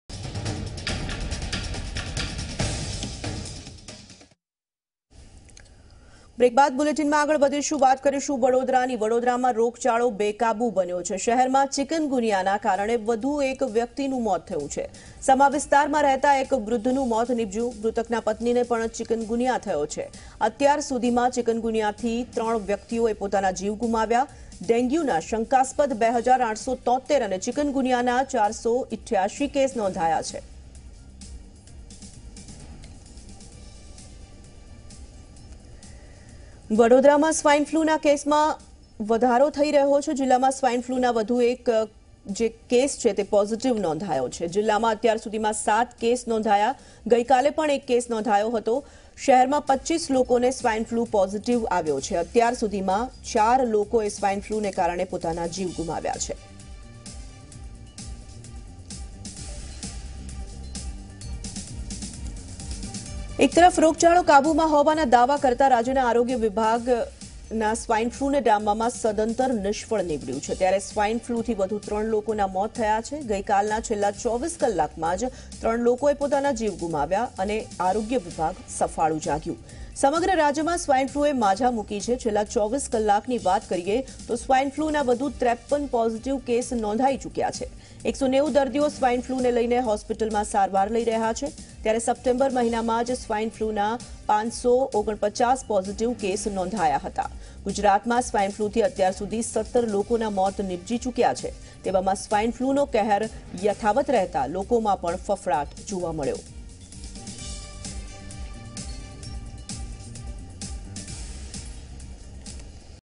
Epidemic in Vadodara, people died due swine flu and chikungunya | Zee24Kalak
Epidemic in Vadodara, people died due swine flu and chikungunya